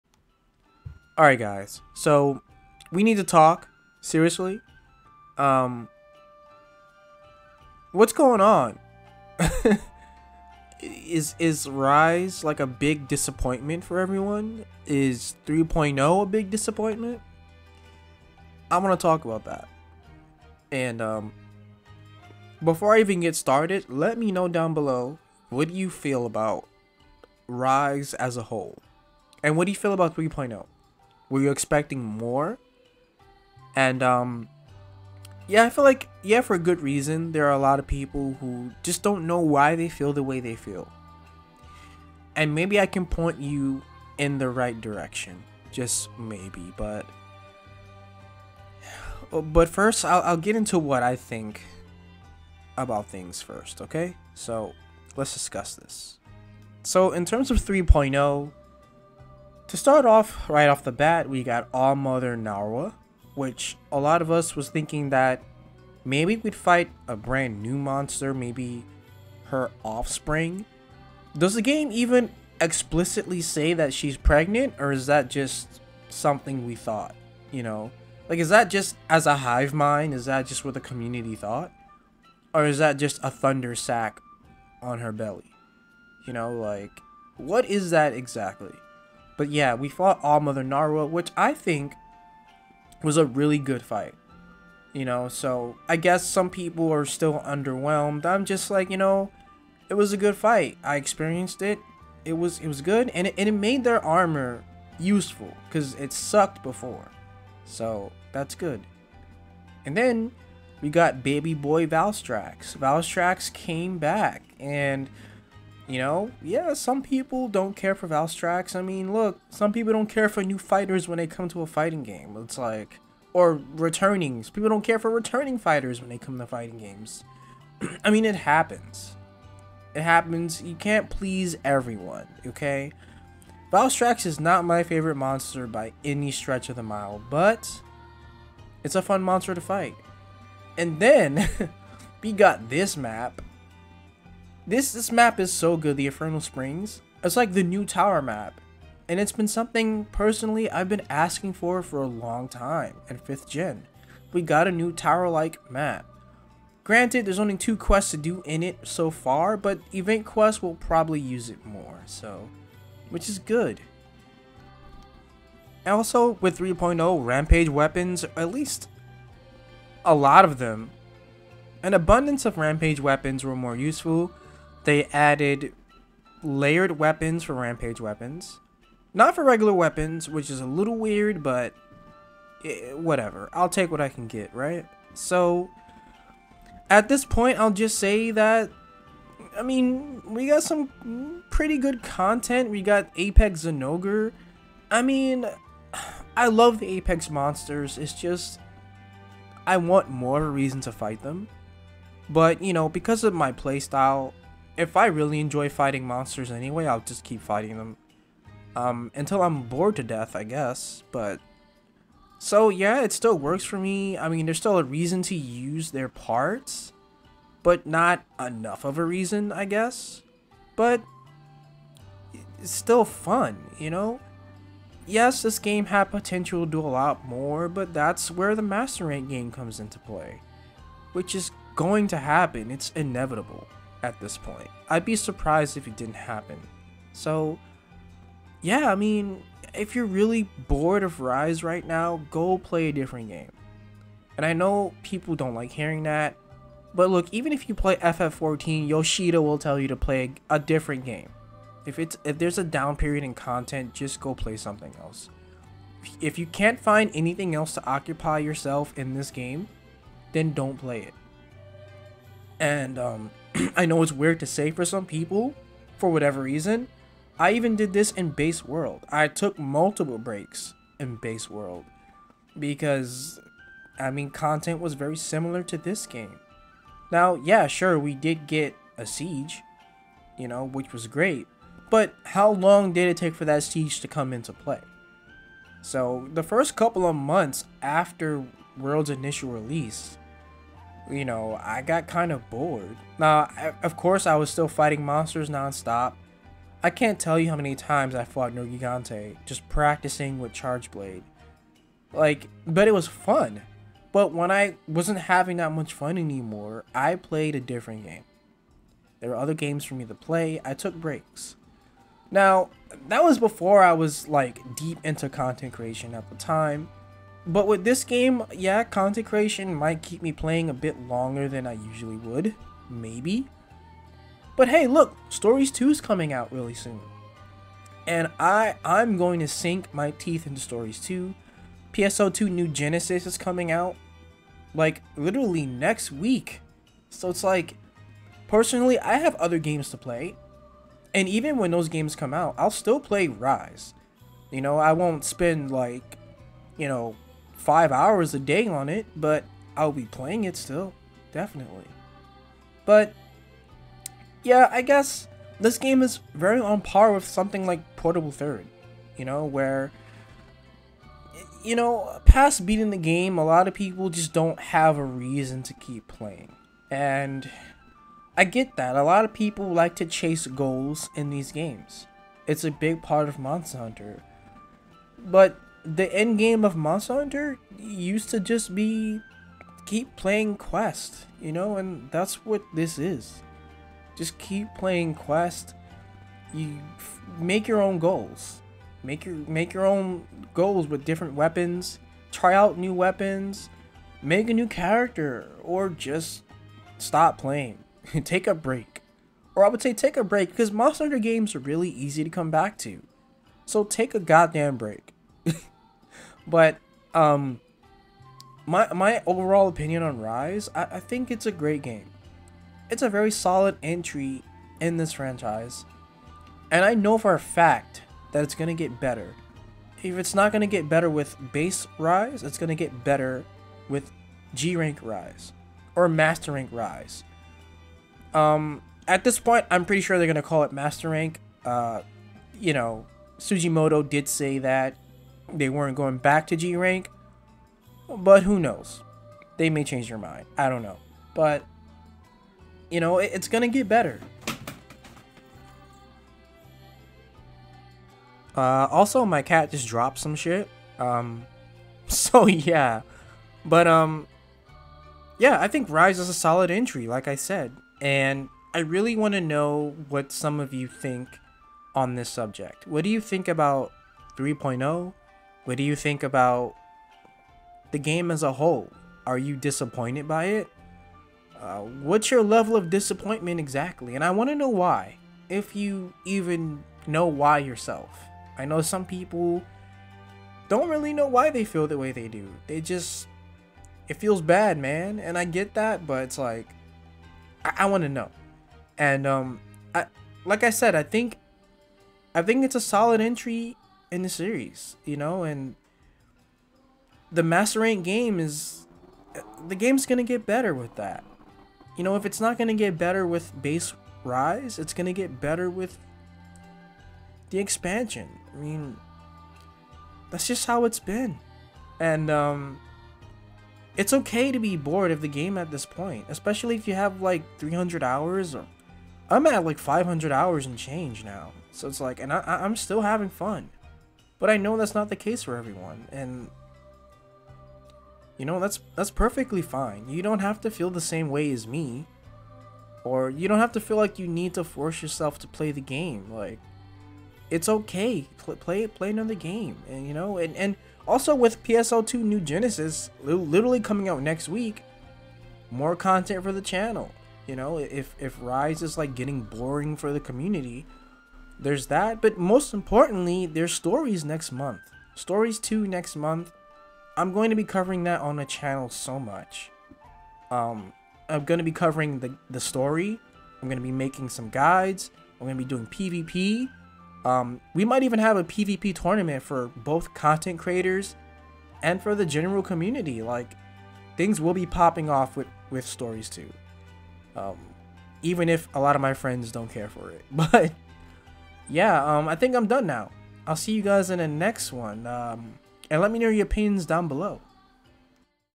Is Rise 3.0 Disappointing!? (Discussion)
R I S E  U P
PLAY MONSTER HUNTER RISE
A lot of people in the community have already burned themselves out
or just straight up bored with Rise. And retiring to FFXIV